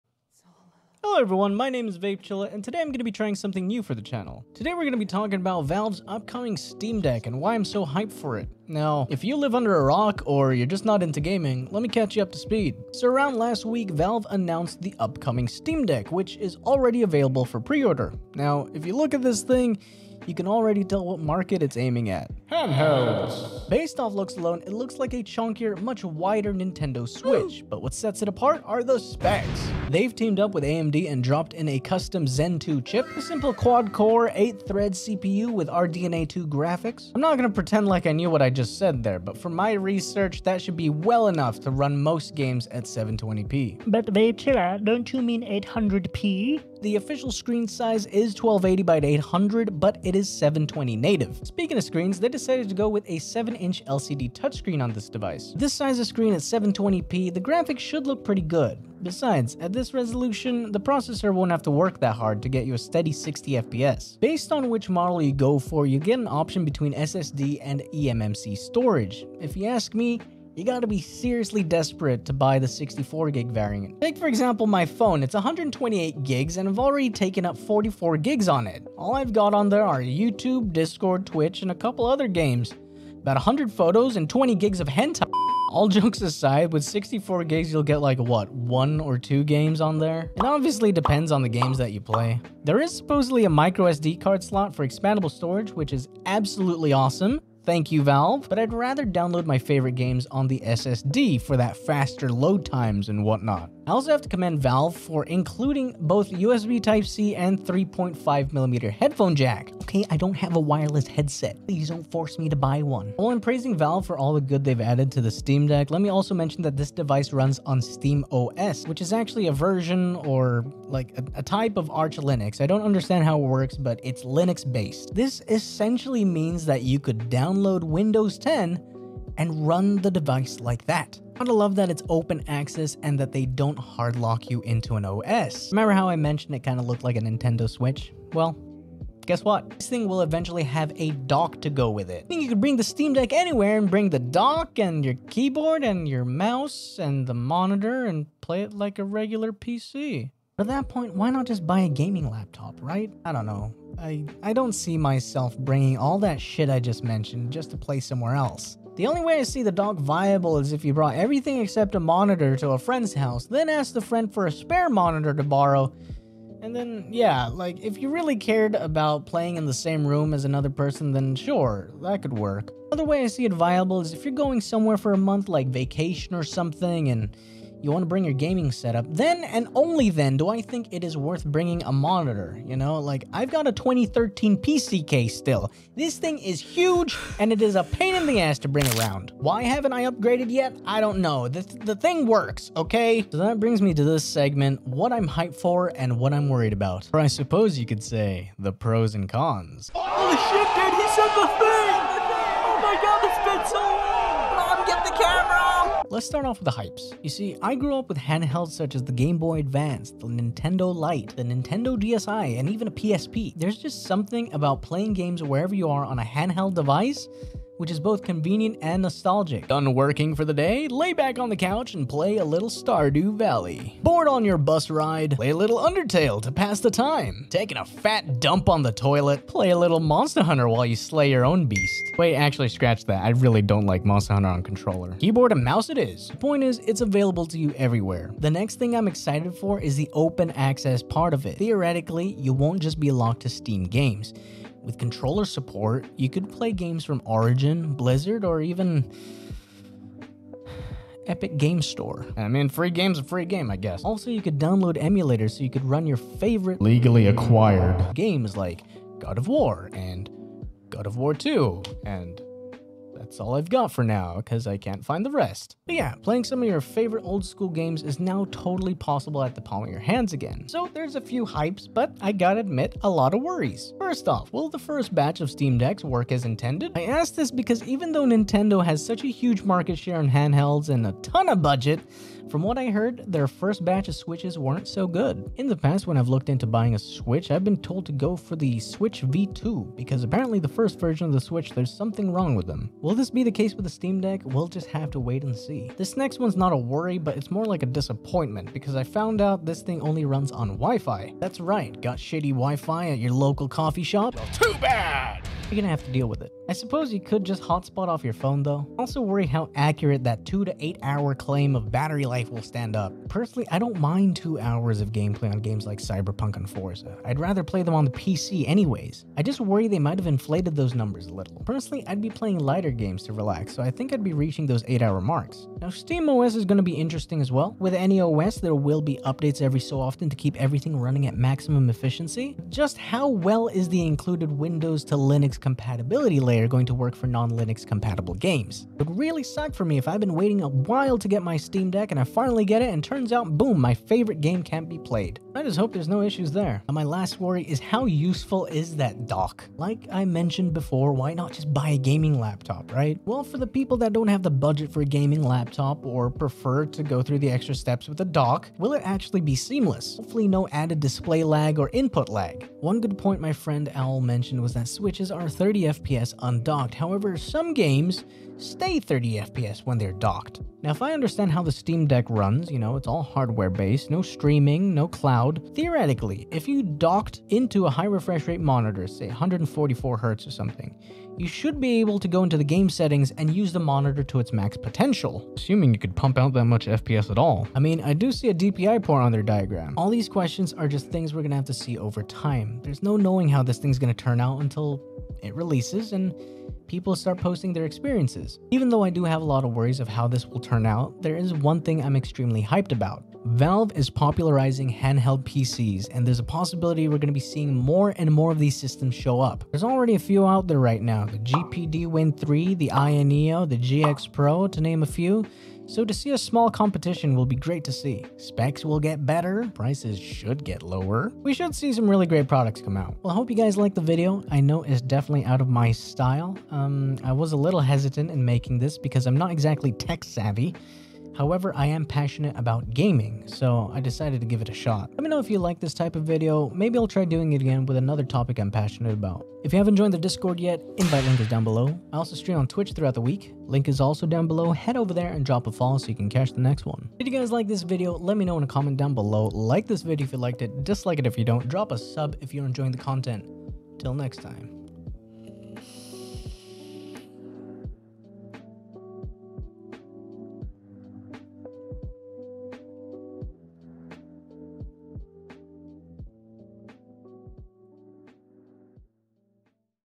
Let's Talk: Valve's Steam Deck
So I'm definitely late on this topic, but I still wanted to make a video on this thing. Introducing possible a new video style: Let's Talk! Valve's Steam Deck looks extremely promising but here I talk about specs what I'm excited for and what I'm worried about. This thing could change the game with handheld PC's! Definitely a challenger against the Nintendo Switch in the handheld market.

Valve is pushing Steam Deck as a fully capable handheld PC, with web browsing, streaming video and access to all your favourite productivity programs as well as games. It'll even let you access rival digital stores, so you're not locked into the Steam library.

0:00 - Intro
0:30 - CPU
0:43 - Screen
1:04 - Storage
1:53 - SteamOS
2:20 - Dock
3:13 - Hypes & Worries
3:26 - Hypes
4:31 - Worries
7:03 - Outro